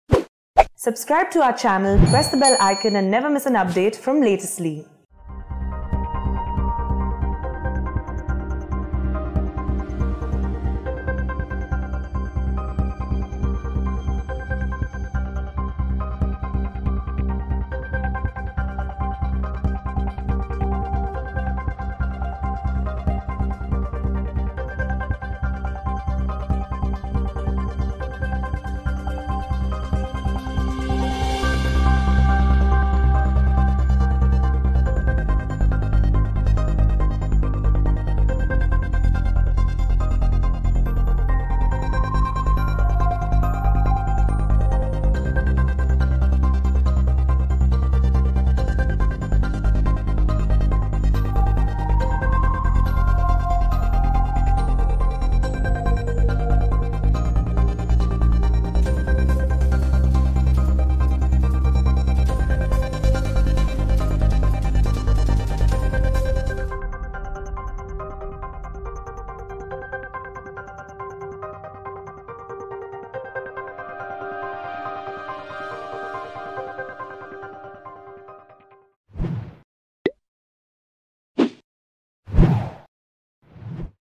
Target Killing: J&K च्या Shopian भागातील Apple Orchid मध्ये कश्मिरी पंडीत भावांवर हल्ला
कश्मीरच्या खोर्‍यात अजून एक कश्मीरी पंडित दहशतवाद्यांचं लक्ष्य ठरला आहे. जम्मू कश्मिर मधील Shopian येथे Chotipora च्या apple orchid मध्ये राहणार्‍या दोन भावांवर गोळ्या झाडण्यात आल्या आहेत. गोळीबारामध्ये एकाचा मृत्यू झाला असून एक जण जखमी अवस्थेमध्ये असल्याची माहिती पोलिसांकडून देण्यात आली आहे.